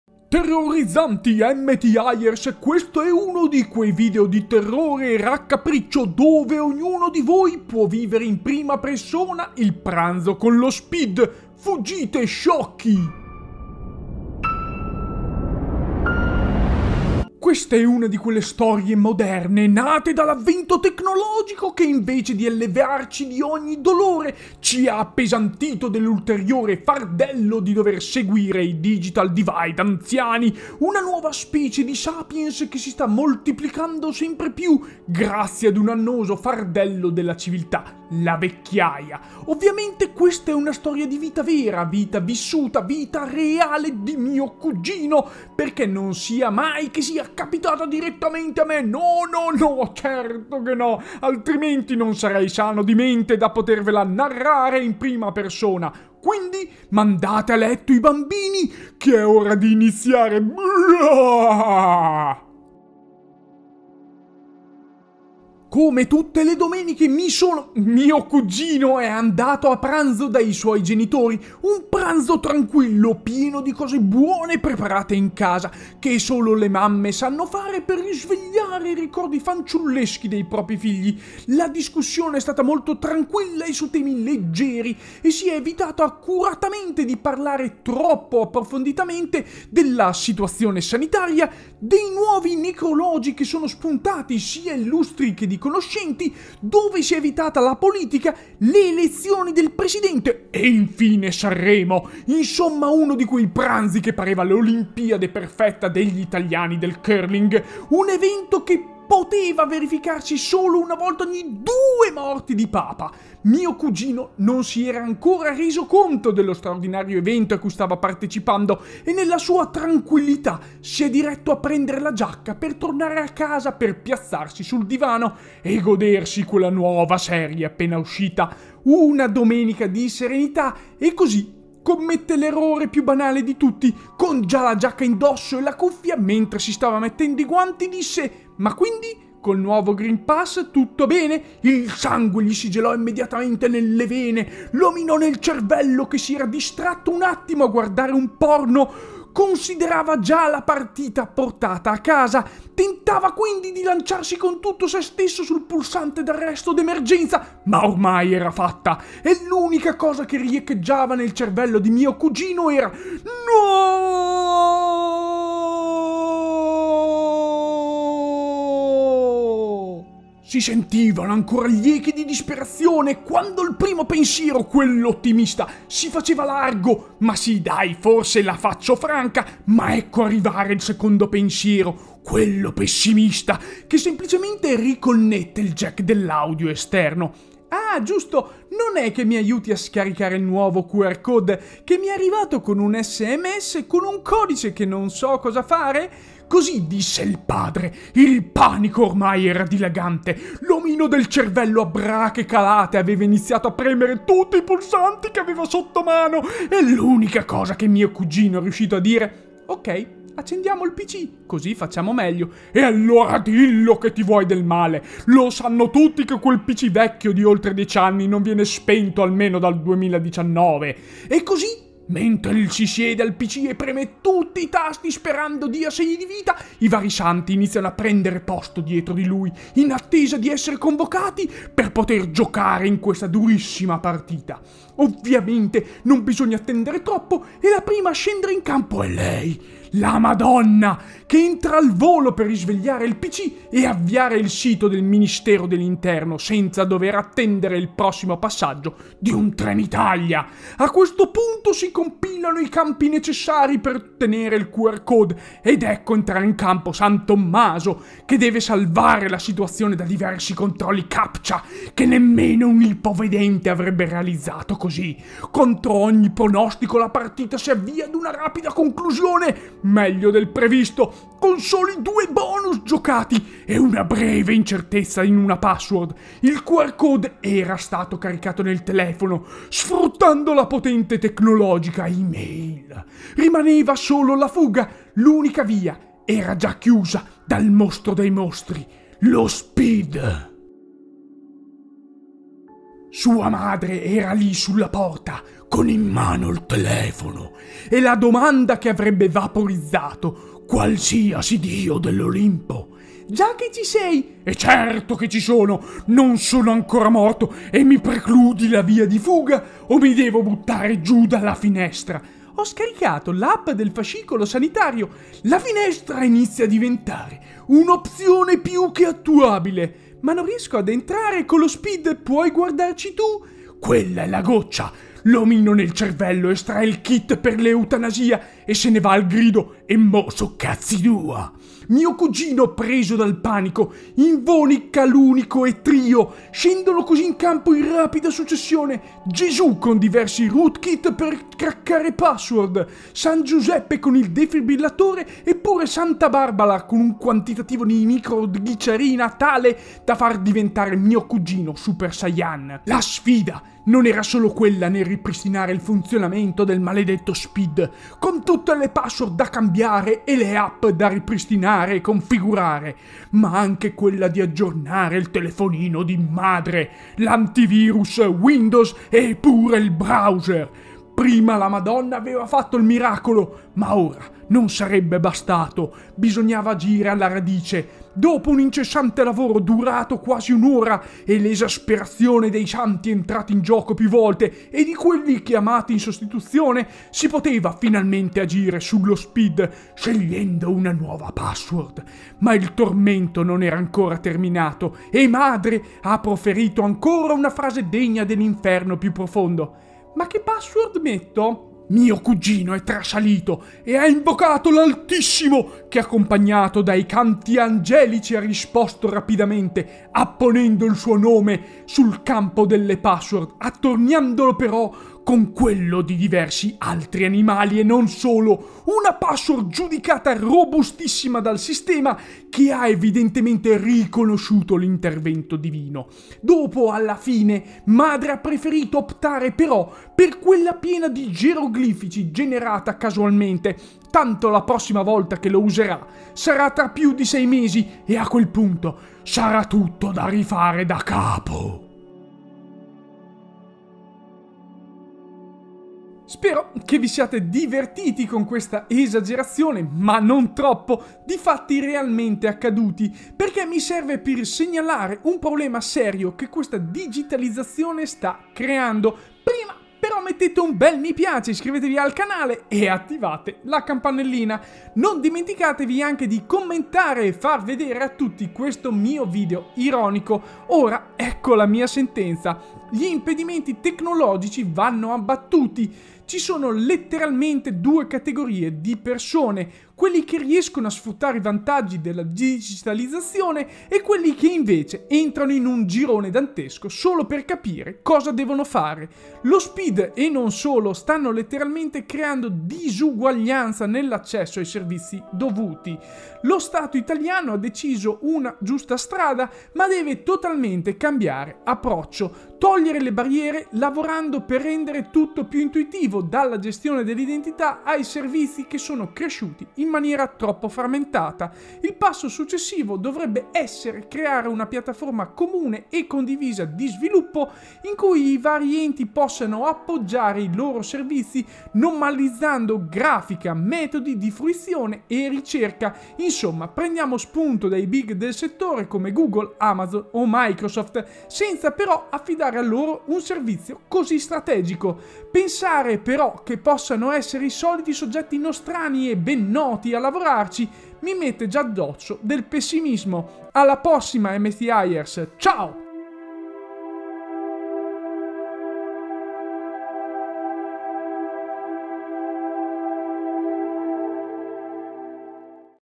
Pranzo in famiglia si trasforma in un incubo
| Argomenti |
0:00 Introduzione
0:17 Storia vera
1:00 Il pranzo
2:45 Il Green Pass
4:49 Lo SPID
7:22 Cosa ci resta